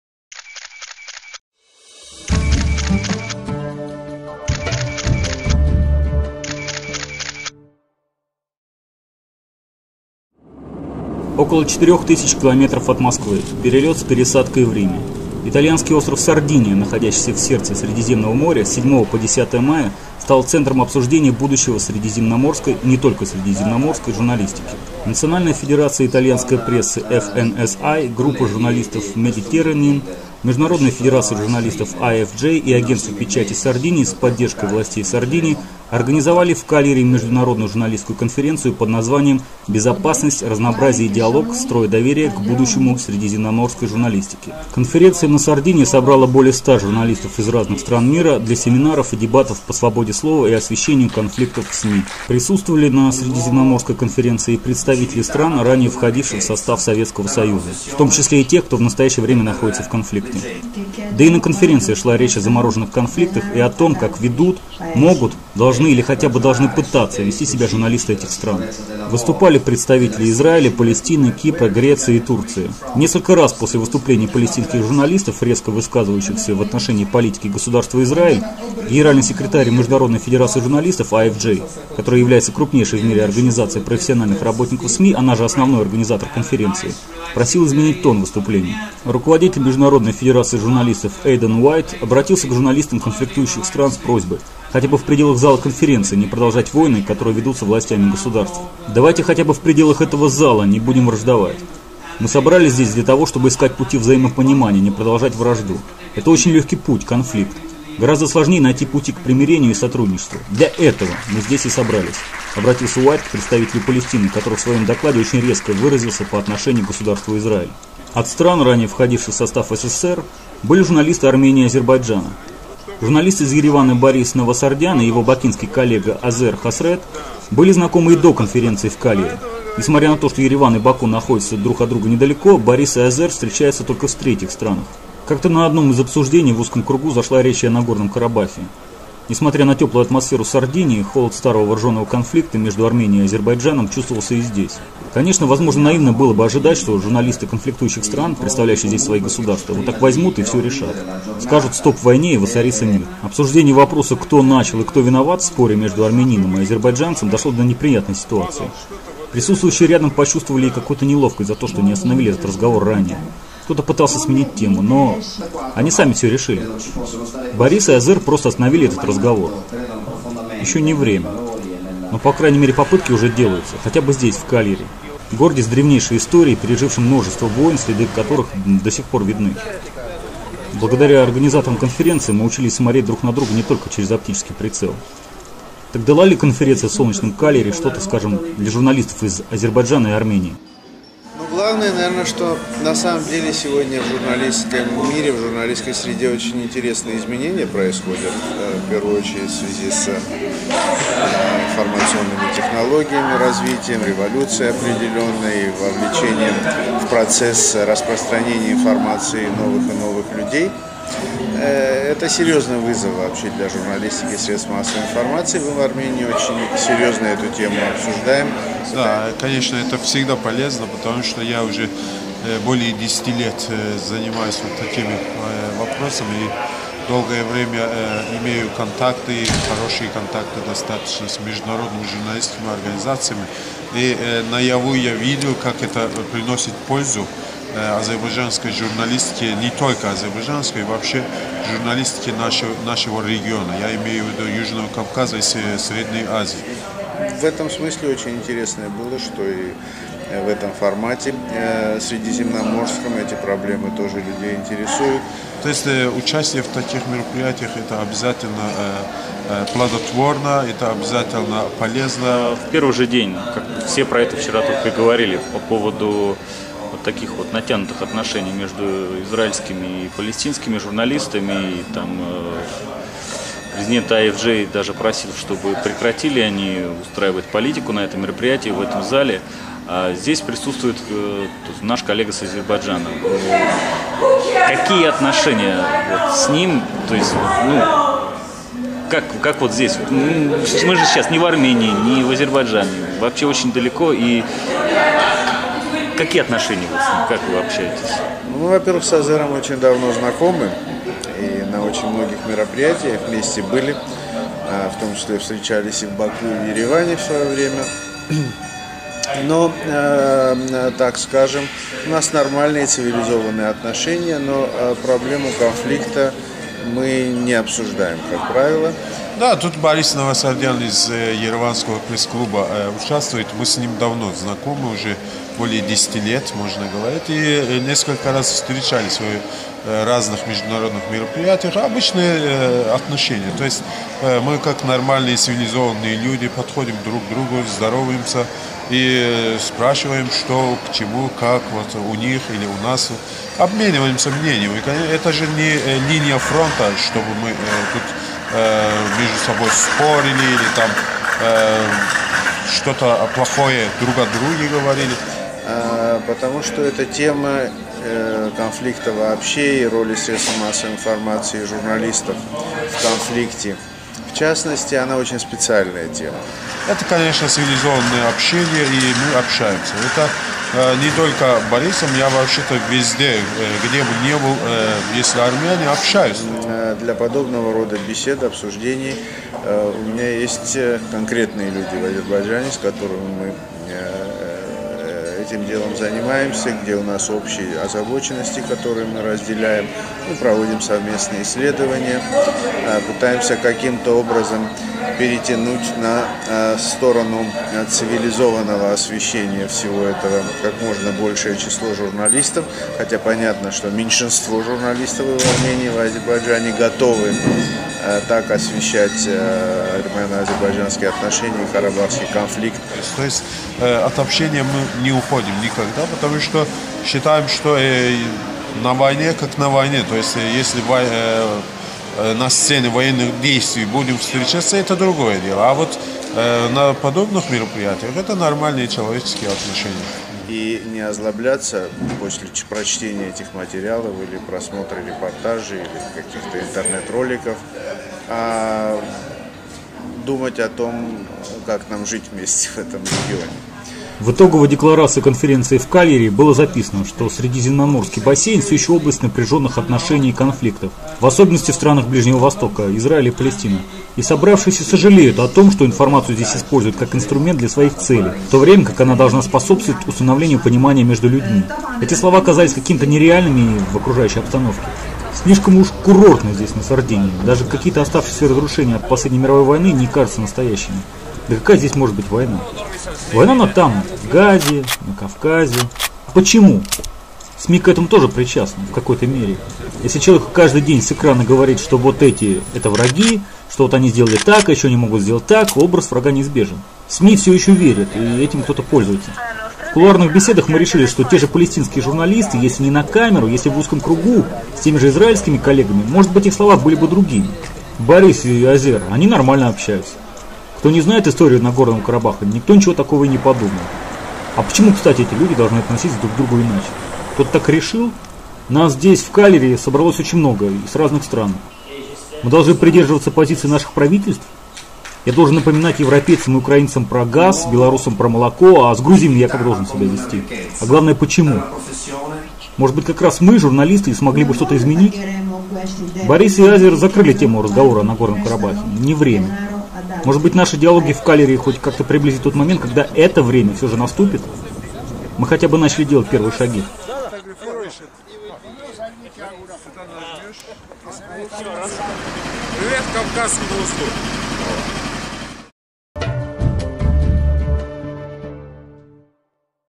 Диалоги о доверии в СМИ
Италия. Сардиния. Кальяри. Май 2010. Автор: Дмитрий Флорин.

В итальянском городе Кальяри, на Сардинии, с 7 по 10 мая состоялась международная журналистская конференция: "Безопасность, разнообразие и диалог, строя доверие -- к будущему средиземноморской журналистики".

Национальная федерация итальянской прессы (FNSI), Группа журналистов Mediterranean, Международная федерация журналистов (IFJ) и Агентство печати Сардинии с поддержкой властей Сардинии собрали более 100 журналистов со всех стран мира.

Присутствовали на конференции и представители стран, ранее входивших в состав СССР. В том числе и тех, чьи правительства в настоящее время находятся в конфликте.
От бывшего СССР в Кальяри приехали журналисты из Грузии, Белоруссии, Молдовы, Украины, Армении, Азербайджана и России.

Журналист из Еревана Борис Навасардян и его бакинский коллега Азер Хасрет были знакомы и до конференции в Кальяри. Несмотря на то, что Ереван и Баку находятся друг от друга недалеко, Борис и Азер встречаются только в третьих странах.
О Нагорном Карабахе предпочитают не говорить. Однако оба уверены, что поддерживать нормальные человеческие отношения, помимо профессиональных, необходимо. Нормальное человеческое общение между армянским и азербайджанским журналистами вселяет надежду на то, что подобные международные журналистские встречи все же имеют смысл.